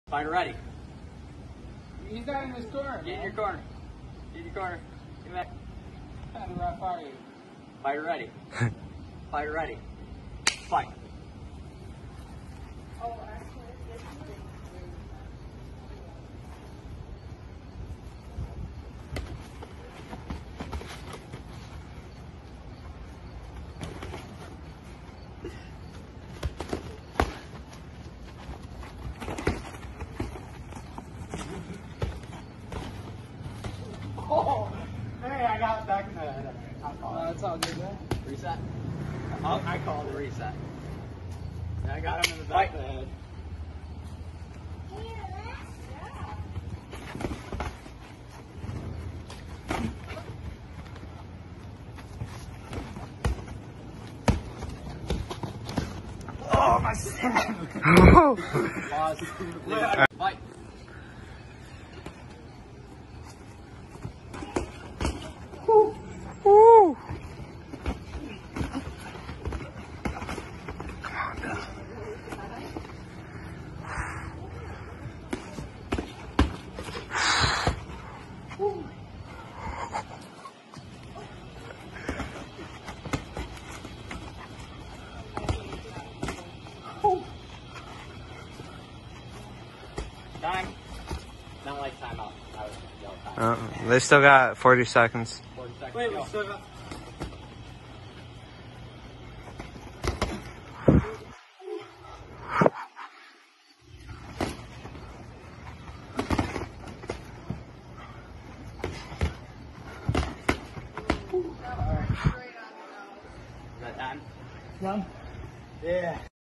Dylan vs Anthony : Boxing
Dylan in the grey/red shorts : 155lbs

Anthony in the black : 210lbs